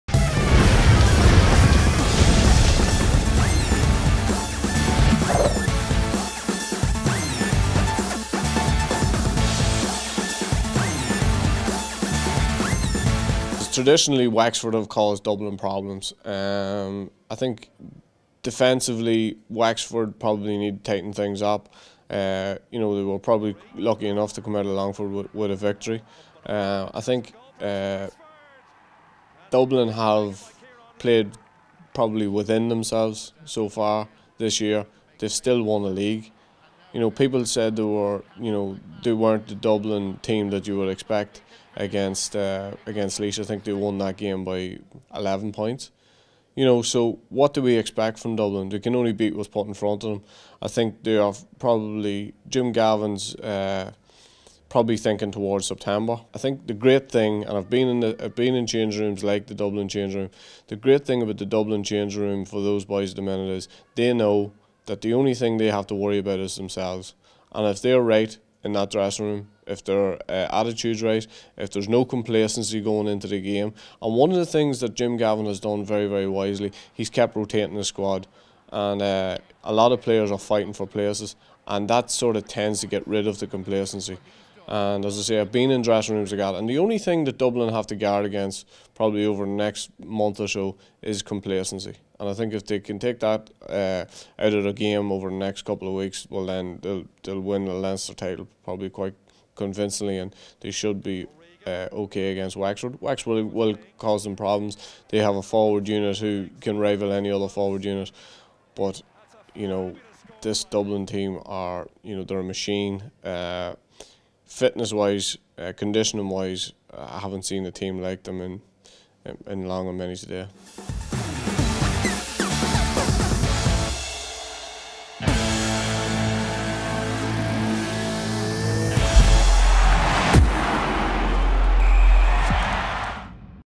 Dublin v Wexford Preview
Hot favorites Dublin continue the defence of their Leinster title against a Wexford side that has run them close in many recent clashes. GAA.ie Columnist Oisín McConville believes complacency is the main threat to the Blue's title charge despite his admiration for the Wexford forward line. The sides meet for the fourth time in five seasons, having previously clashed in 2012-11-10. Wexford are seeking their first championship win over Dublin since the 1956 Leinster semi-final which they won by 2-7 to 0-7 in Carlow. Since, then Dublin have beaten Wexford eighteen times, the latest coming in 2012 when then won a Leinster semi-final by 2- 11 to 1-10 on day when Kevin McManamon (1-3) and Diarmuid Connolly (1-1) were Dublin's top scorers. Ben Brosnan (0-4) and Redmond Barry (1-1) were Wexford's top scorers. Dublin, chasing a four-in-a-row in Leinster, are bidding to reach the final for the 9th in ten seasons (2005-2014), having missed out only once in 2010 when they lost the semi-final to Meath. Wexford who last won the Leinster title in 1945, last reached the final in 2011, losing to Dublin.